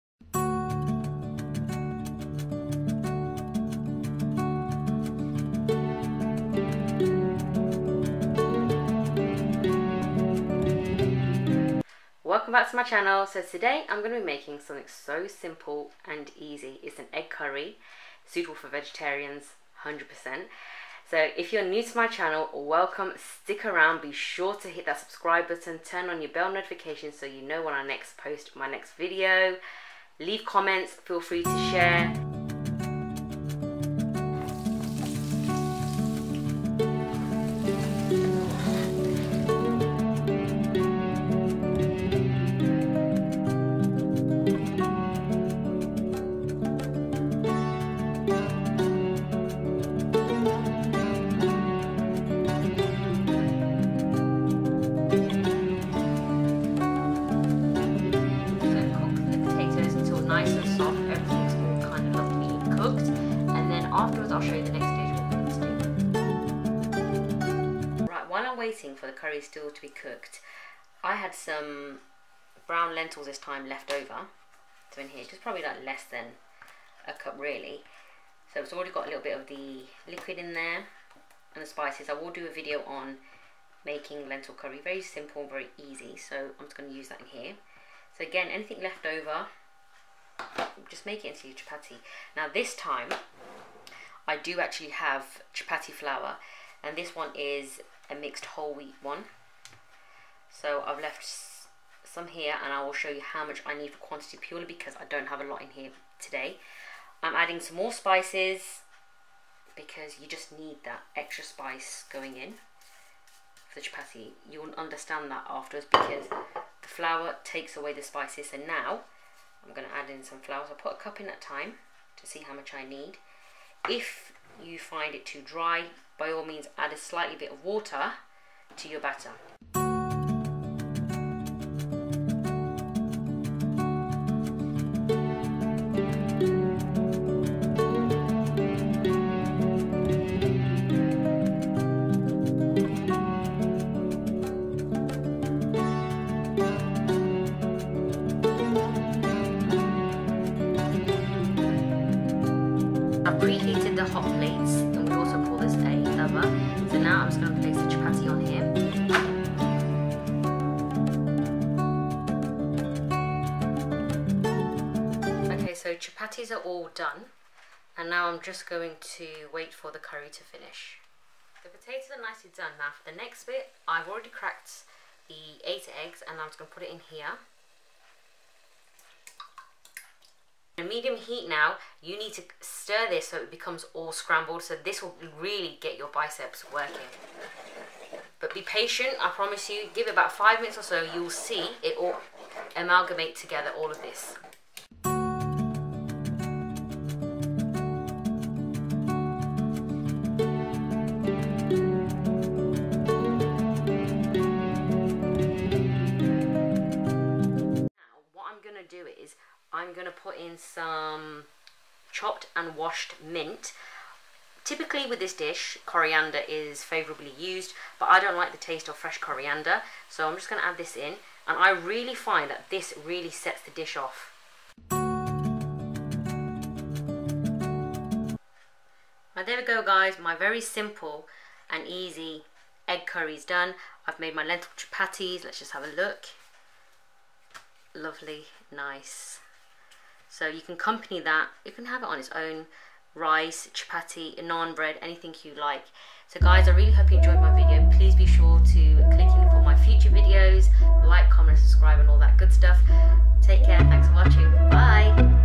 MAKING EGG CURRY | ALOO ANDAY SUPER EASY
Hi all
In this video, I am making an Egg and potato curry suitable for vegetarians (providing you eat egg.) The name maybe known as Aloo anday or Anday aloo.
Recipe is listed and the quantity of spices can vary and be reduced to your specific requirements.
thanks x

snap chat:   syma1